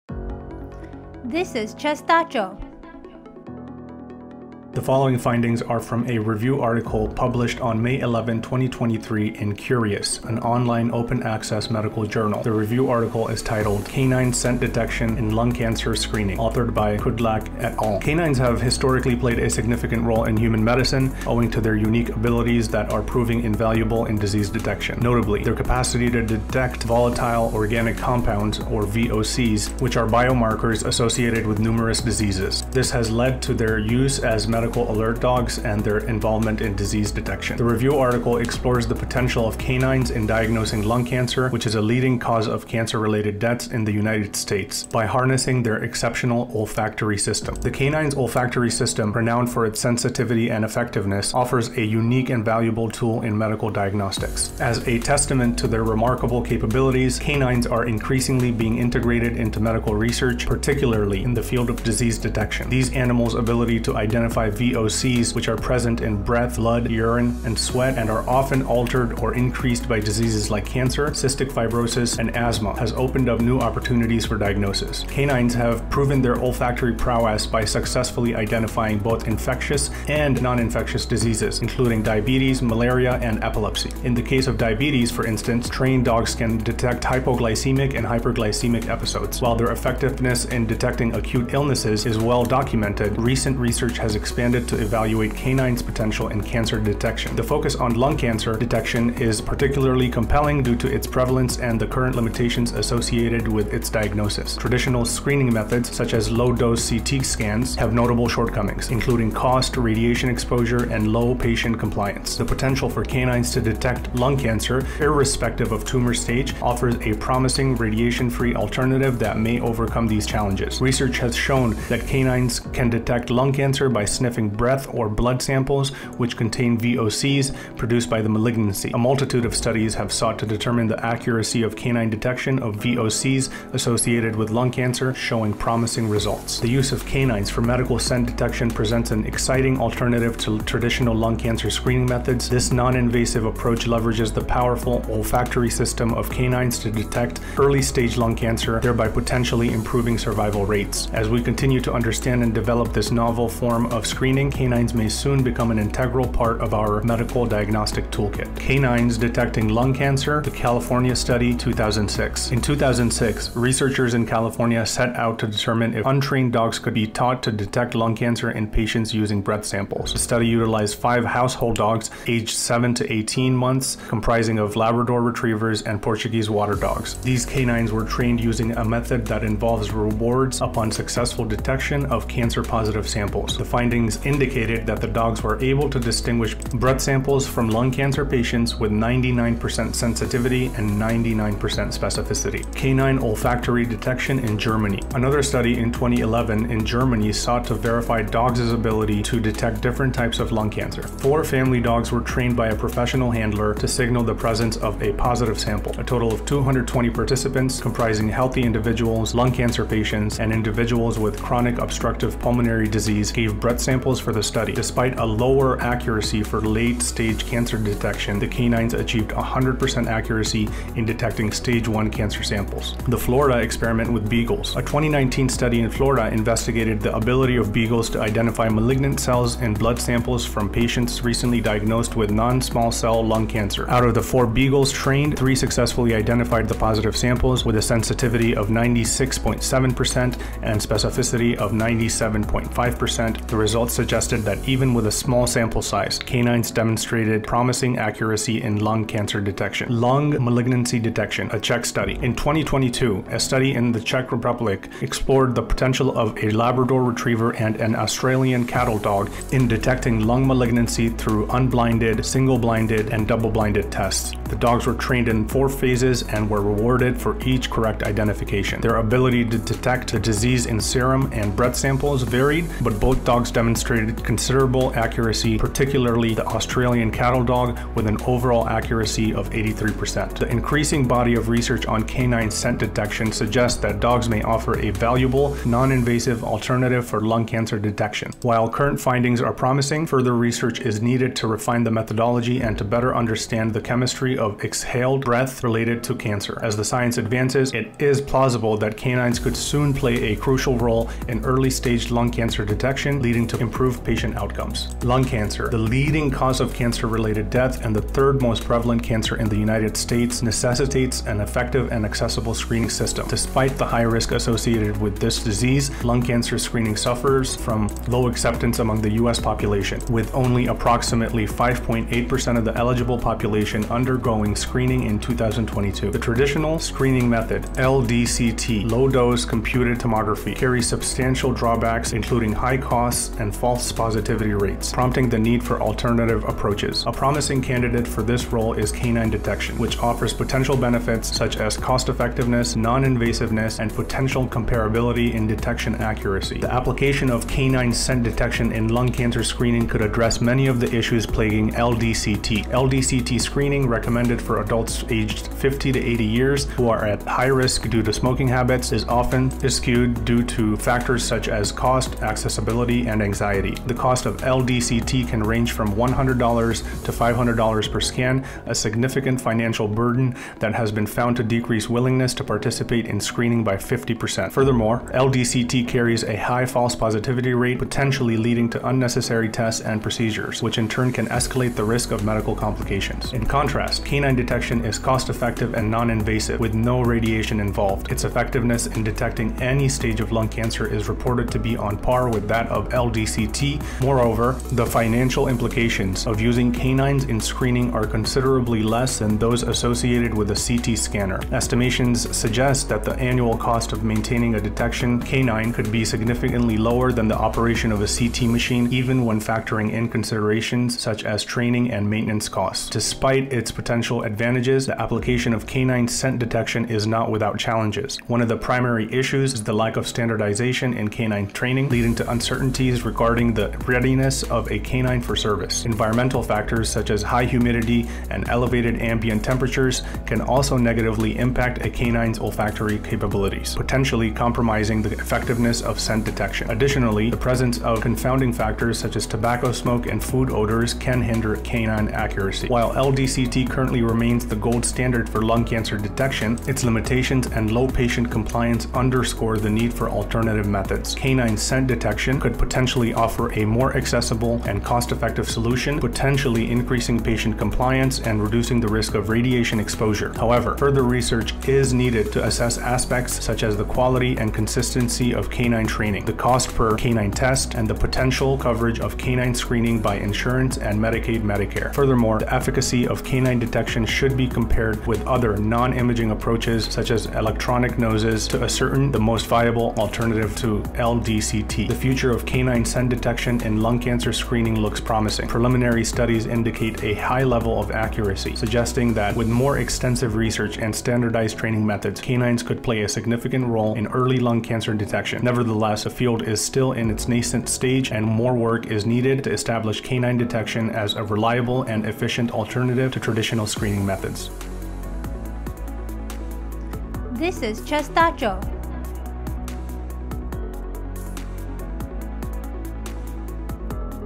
Dogs can smell cancer, epilepsy, diabetes, and more. The Dogtor can see you now!
Explore the promising future of medical scent detection in this captivating video essay! Based on a study by Kudlak et al., published in the open access medical journal Cureus on May 11, 2023, we delve into the potential of using canines in early lung cancer detection. Despite the gold-standard status of Low-Dose Computed Tomography (LDCT), patient compliance remains low due to various concerns such as cost, false positivity, and screening-related anxiety. Canine scent detection presents a fascinating alternative, offering cost-effectiveness, decreased test-related anxiety, and possibly increased patient compliance. Join us as we navigate the nuances of this innovative approach and discuss the areas that need further research.

Kudlak M, Ali M, Whitlow Kirk S, et al. (May 11, 2023) Canine Scent Detection in Lung Cancer Screening. Cureus 15(5): e38877. doi:10.7759/cureus.38877